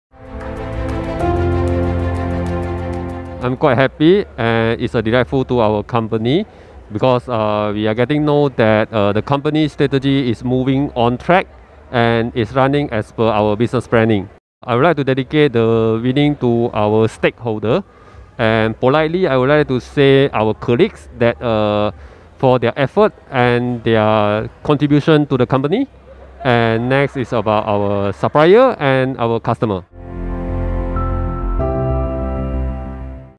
CLPG PACKAGING INDUSTRIES SDN. BHD. - 2022 Winner of SME100 SAMENTA 35th Anniversary Edition
Congratulations to the winners of 2022 SME100 SAMENTA 35th Anniversary Edition!

The Northern Malaysia-region SME100 awards received overwhelming response from 178 companies from Penang, Kedah, Perak, and Perlis SMEs, submitting their nominations, exceeding the organisers’ target of 100 nominations.